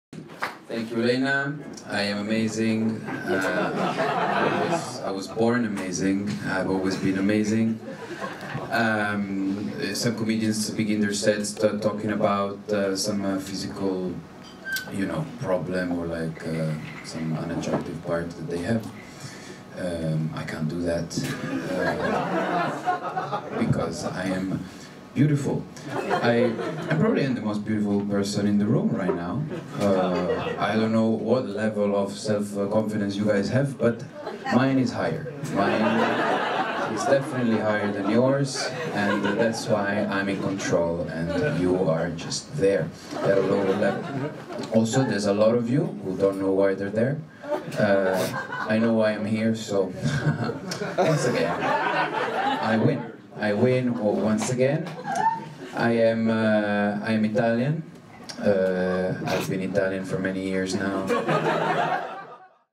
Filippo Spreafico is better than you @ The Last Stand, Berlin
Leather-jacketed Filippo Spreafico once again wins against an audience made of people ***WARNING hard subtitling***